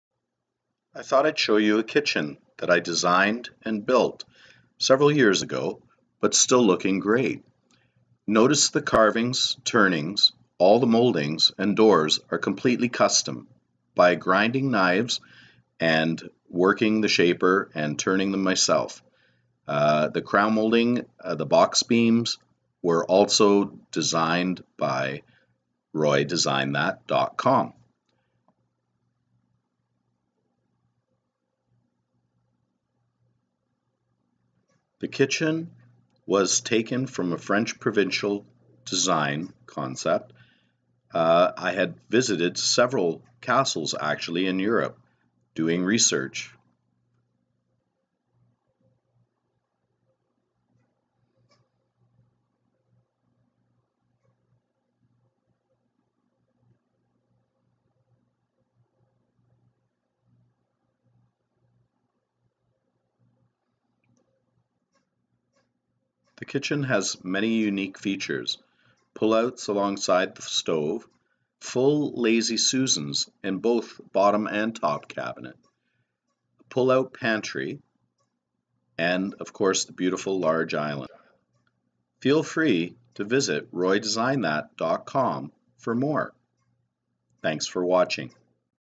Custom Kitchen designed and built by RoyDesignedThat.com
An example of a kitchen design by RoyDesignedThat.com